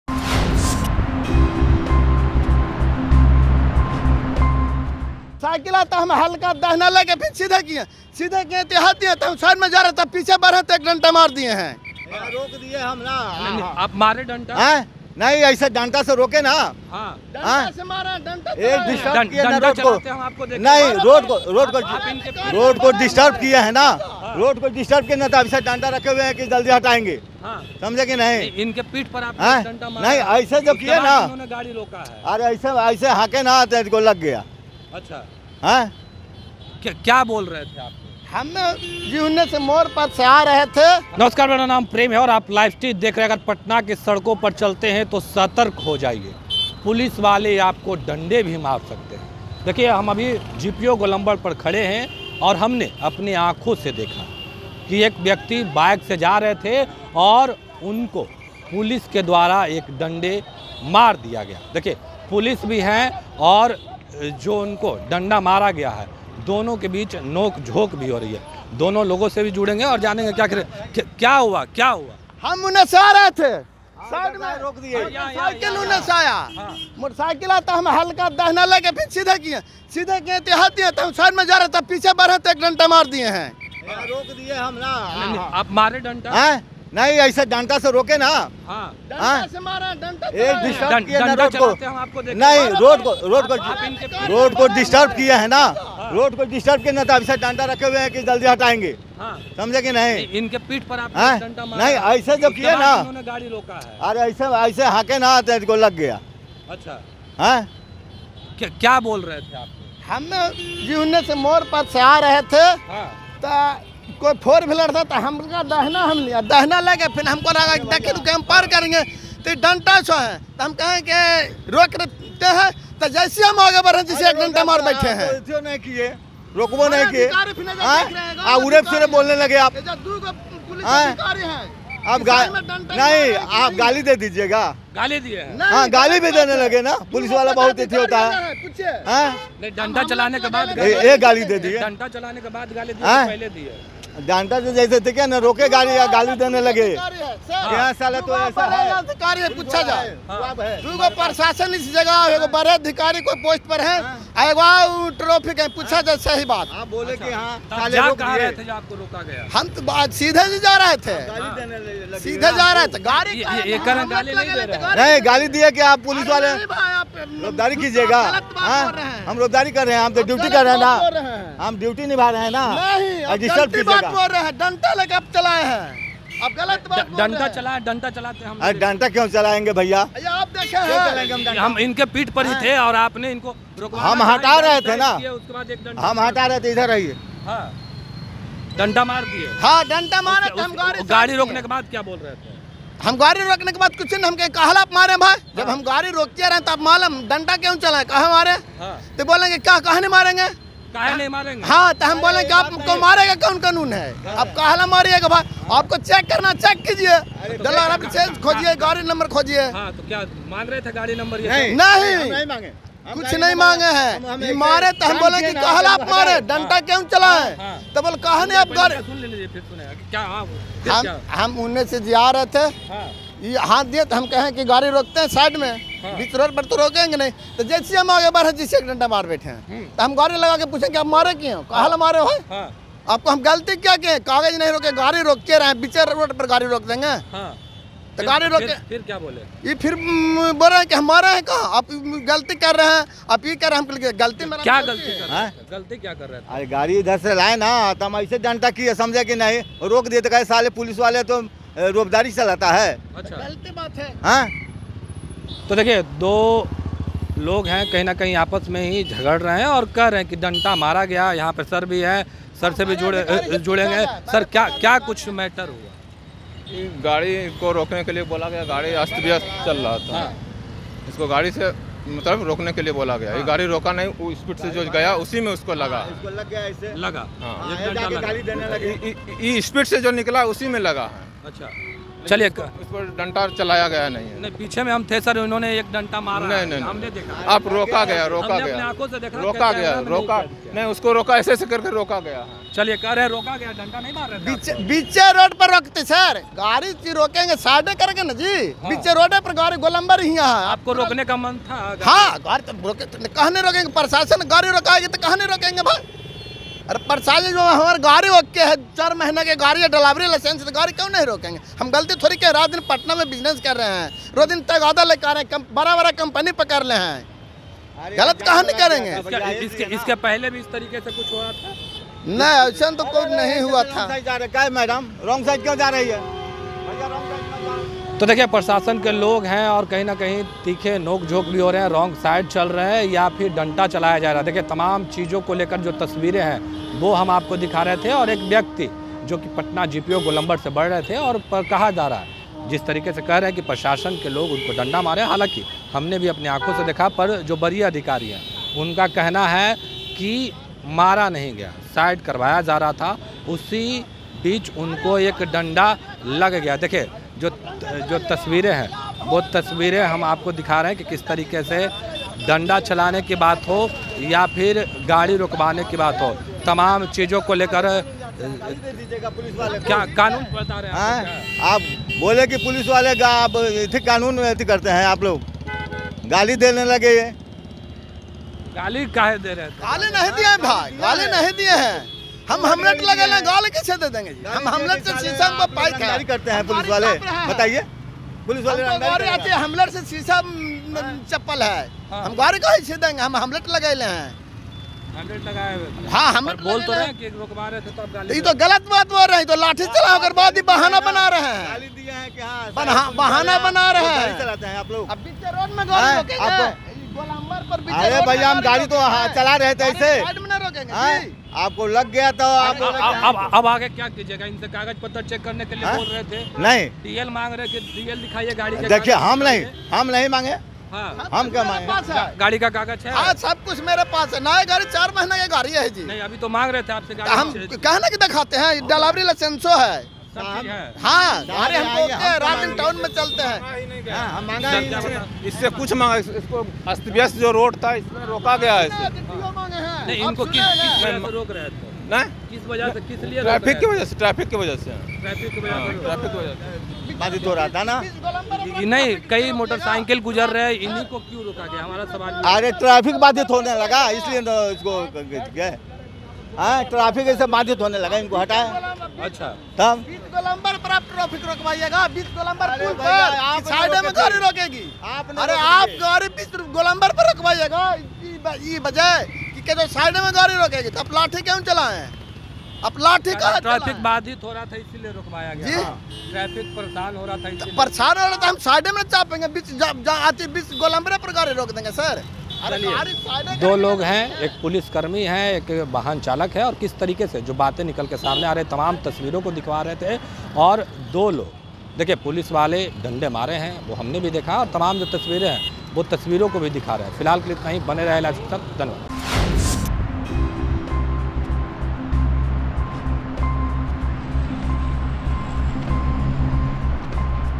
Patna में बीच रोड पर Traffic Police से भिड़ गया बाइक वाला, गाड़ी रोकते ही टूट पड़ा, पीठ पर काहे मारा
Patna में बीच रोड पर Traffic Police से भिड़ गया बाइक वाला, गाड़ी रोकते ही टूट पड़ा, पीठ पर काहे मारा... | Bihar News | LiveCities